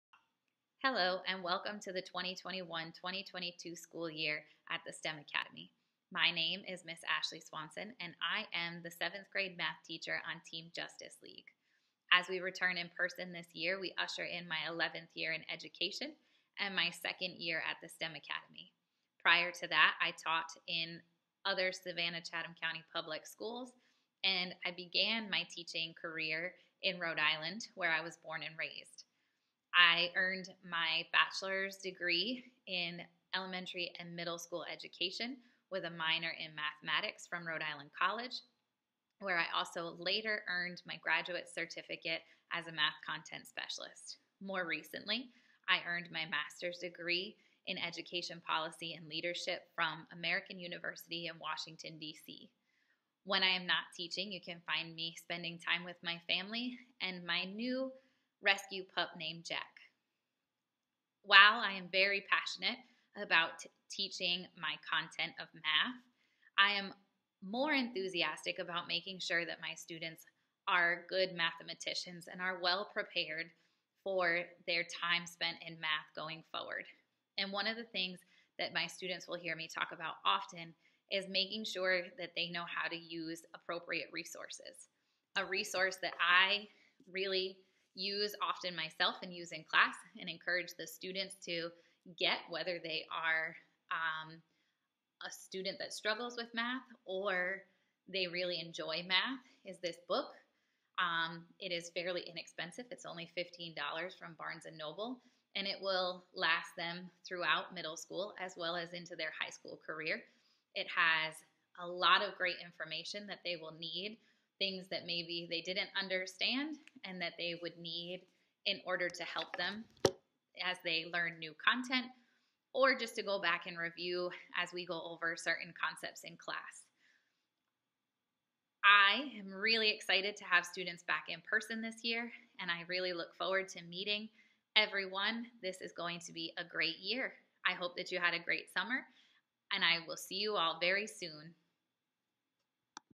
WelcometoSTEMSAV - Meet Ms. Swanson
Welcome to The STEM Academy! Meet Ms. Swanson, part of the Seventh Grade team.